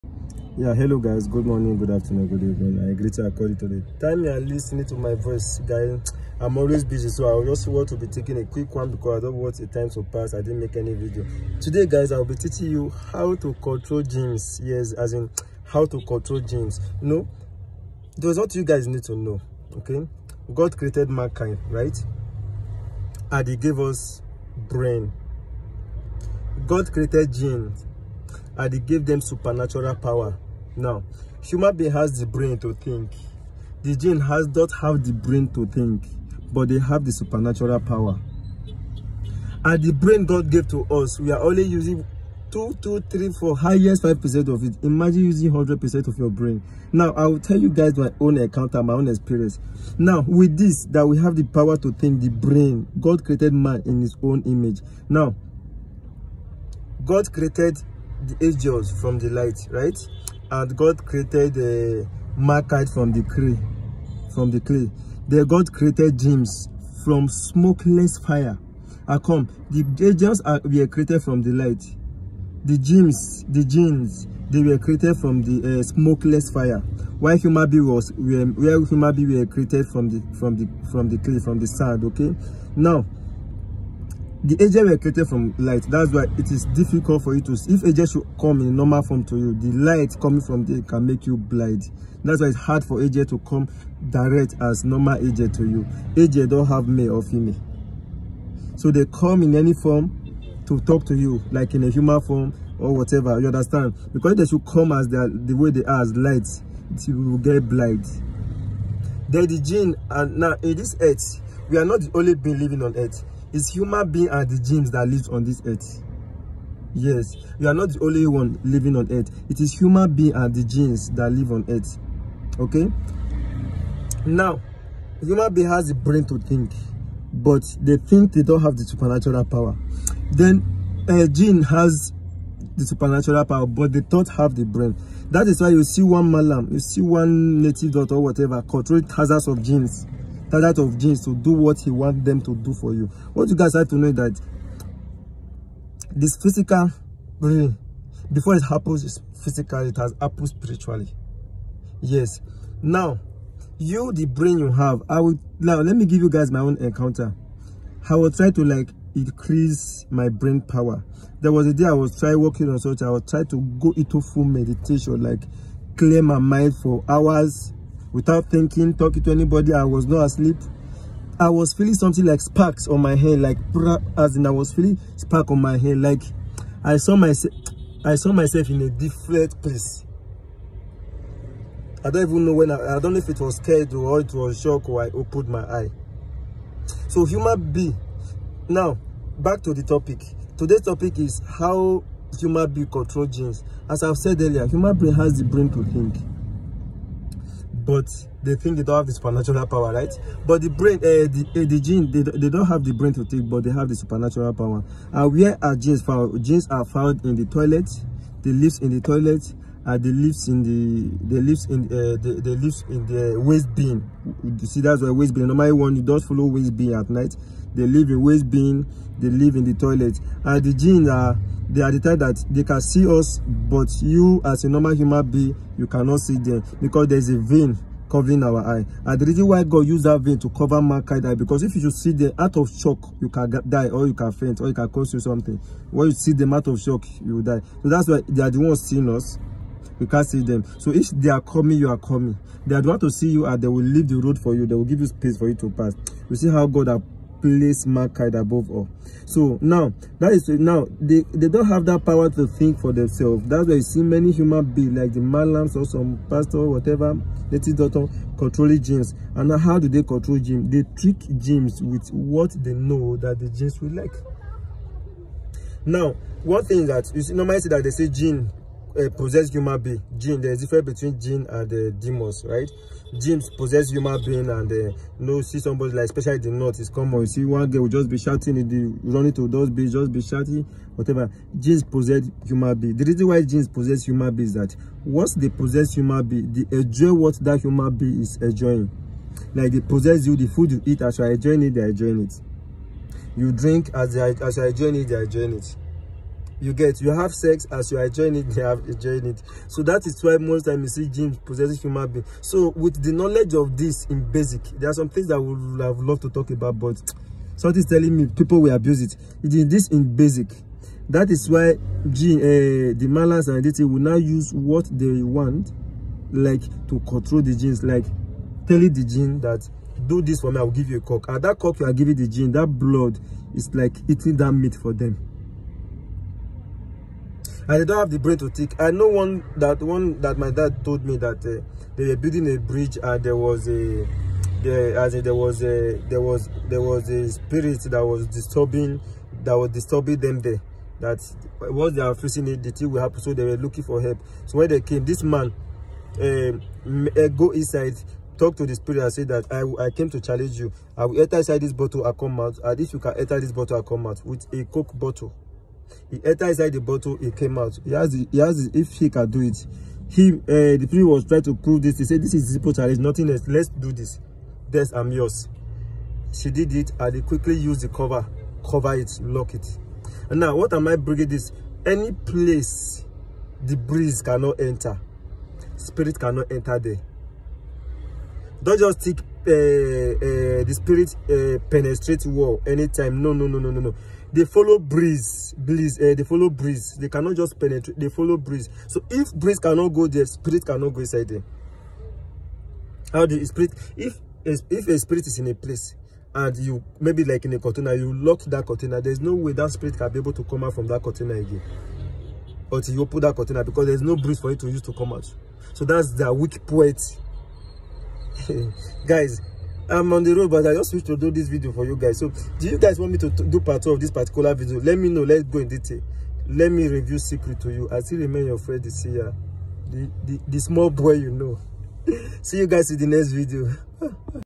How to control Jinns(Spirits)to do anything you want them to do for you
With brain God gave to mankind,humans have the ability to tricks Jinns to work for them